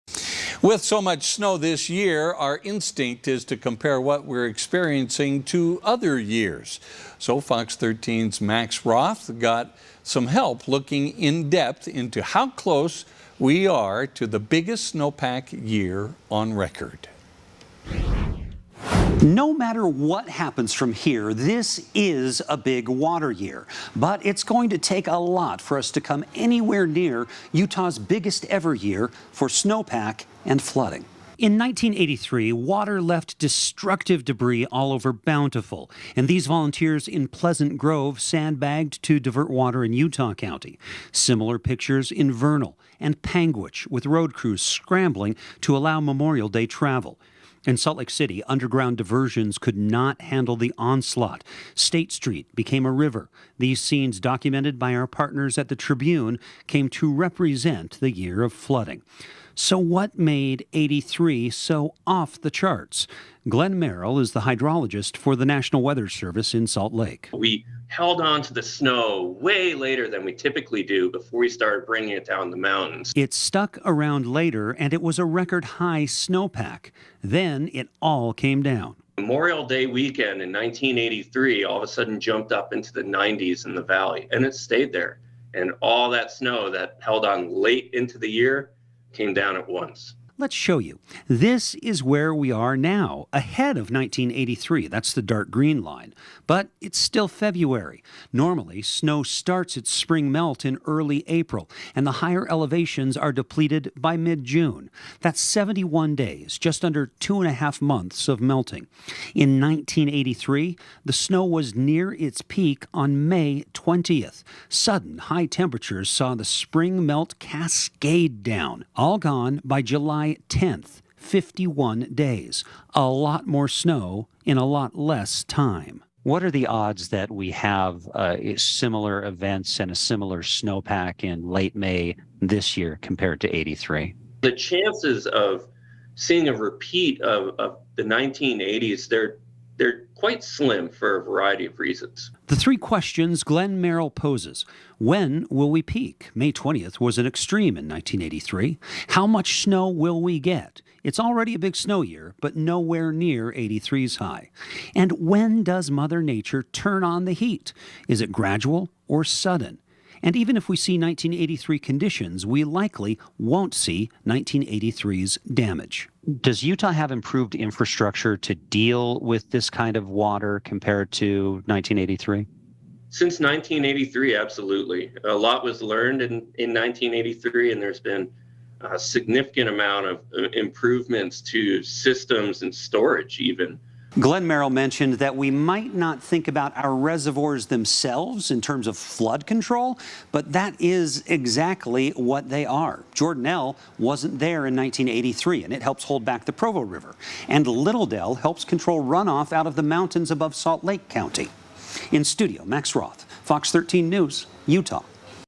How does this year's snowpack compare to 1983's runoff catastrophe?
It's been a good winter for Utah's snowpack. But how does it compare to other years?